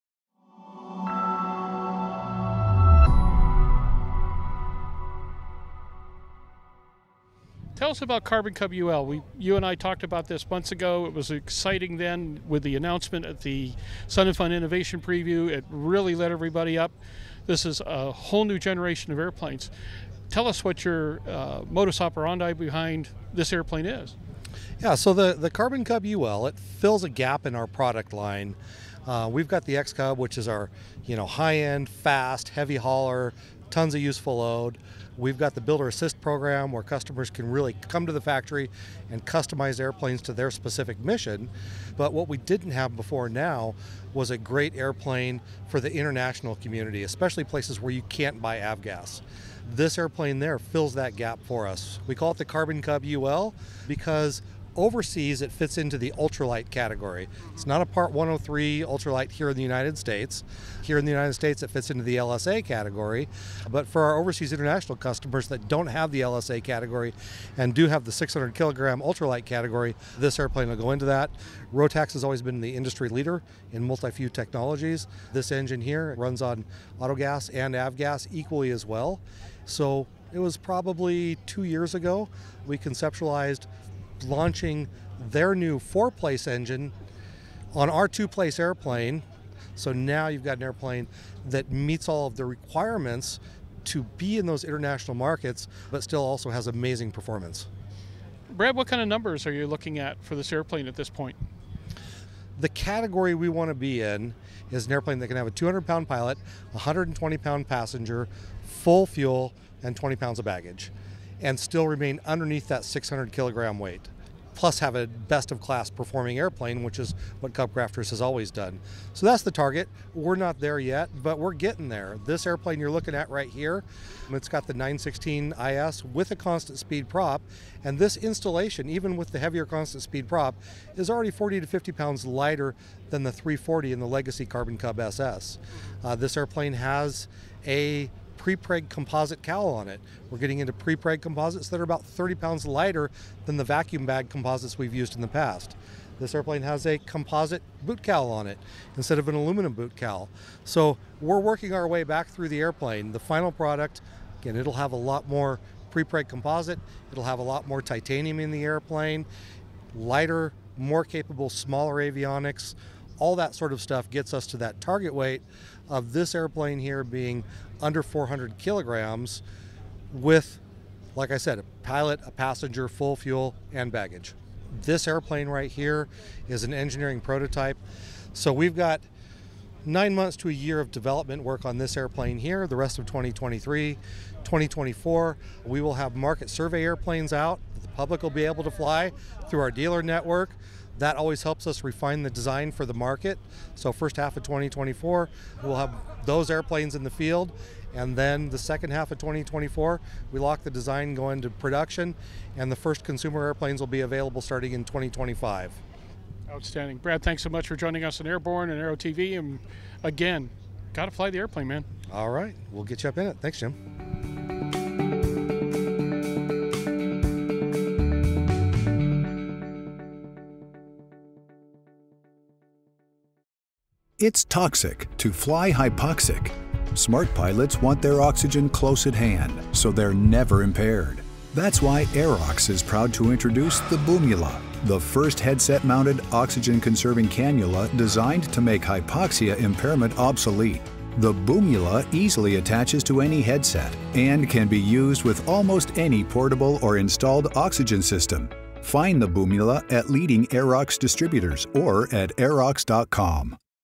CubCrafters Carbon Cub UL Dazzles SUN ‘n FUN Attendees
Less is More; More is More

CubCrafters has introduced a new iteration of its best-selling Carbon Cub aircraft. Dubbed the Carbon Cub UL, the new machine is intended primarily for international markets in which conventional avgas, the likes of 100LL, is difficult or impossible to come by.

To achieve its goal of making the Carbon Cub UL contemporaneously avgas and mogas compatible—as well as even lighter and better-performing than its Carbon Cub SS predecessor—CubCrafters invested significantly in new technologies. The end result is an aircraft that complies fully with rigorous American Society of Testing and Materials (ASTM) standards and carries two adults, full fuel, and a reasonable amount of baggage at a takeoff weight of 1,320-pounds (six-hundred kilograms).

Among the instances salient to the Carbon Cub UL’s development was CubCrafters’s collaboration with BRP-Rotax, the Austria-based, Bombardier-owned engine marque renown for its sophisticated-but-reliable, low-weight, high-power reciprocating-engines. The Carbon Cub UL is the launch platform for Rotax’s new 160-horsepower 916 iS powerplant, a remarkable turbocharged mill capable of producing more power than the Carbon Cub SS’s normally-aspirated CC340 engine under high density altitude conditions.

CubCrafters president and CEO Patrick Horgan stated: “The Carbon Cub was first introduced in 2009 and has been an amazing aircraft for both CubCrafters and the entire backcountry community. It has redefined expectations industry-wide for what a light STOL aircraft should be. Now, we’re reimagining the Carbon Cub by incorporating the latest in prepreg composites, more titanium components, and innovative manufacturing techniques, along with the best new engine and avionics technologies for even more performance and versatility.”

Prepregs are materials applied to the surfaces of aerospace composites for purpose of improving the properties thereof. Prepregs are typically made from glass, carbon, or aramid fibers coated with thermoset resin for additional strength. Prepregs occasion lightweight and robust alternatives to legacy aerospace composites. What’s more, prepregs are environmentally-friendly and may be safely disposed of in landfills.

Mr. Horgan added: “The Carbon Cub UL is the culmination of years of investment in both human and economic resources, the hard work of our industry-leading team, and our dedication to research and development. We believe this aircraft will make a major contribution to unlocking the enormous potential of the international marketplace.”

The production version of the Carbon Cub UL is to be built, certified, and test-flown as a Light Sport Aircraft (LSA) at CubCrafters Yakima, Washington factory. The aircraft will also meet Ultralight category requirements in many international jurisdictions.

CubCrafters vice-president of sales and marketing Bran Damm set forth: “The aircraft can remain in the LSA category for our customers in Australia, New Zealand, Israel, and even the United States, but it can also be deregistered, exported, and then re-registered as an Ultralight category aircraft in many jurisdictions in Europe, South America, and elsewhere. This is a concept we’ve looked at for the last several years. Our kit aircraft program has always been strong in overseas markets, and now we are very excited to have a fully factory-assembled and tested aircraft to offer to our international customers.”

On 14 March 2023, a technology demonstrator embodying many of the engineering concepts intrinsic to the new Carbon Cub UL successfully landed and took off again from a 27-meter diameter heliport atop Dubai’s iconic Burj Al Arab hotel. Designated the Bull’s Eye Landing Event, the feat was executed by Red Bull Air Race pilot and Carbon Cub owner Luke Czepiela, and performed in cooperation with Red Bull and extreme sports promoter XDubai.

The first Rotax 916 iS powered Carbon Cub UL, an engineering development prototype, was displayed at 2023’s SUN ‘n FUN Aerospace Expo. Throughout the spring event, CubCrafters accepted deposits for the new model, the first deliveries of which are slated to occur in early 2025.

Parties interested in learning more about the Carbon Cub UL—to include the aircraft’s engine and airframe technical specifications, a product launch Q&A, and video and still image galleries—are invited

Aero-TV is a production of the Internationally syndicated Aero-News Network.